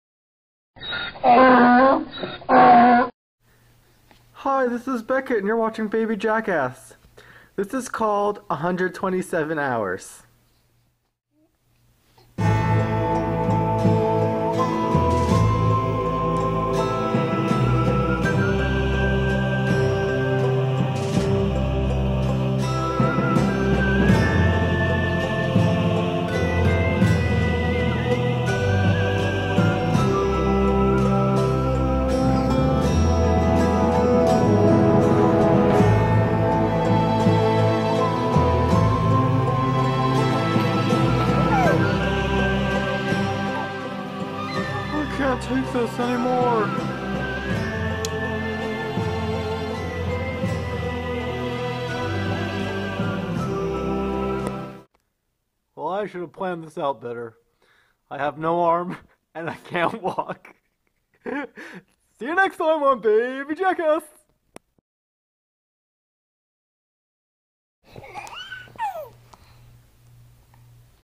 baby jackass - 127 hours
Beckett Sage pays tribute to James Franco paying tribute to Aron Ralston. Can Beckett survive 127 hours in his stroller? Will he be forced to make a life-altering decision? Filmed at Hyatt Grand Champions Resort, Indian Wells, CA

Read Evan’s Dumb Books
Mike Francesa’s Greatest Hits
Dumb Stuff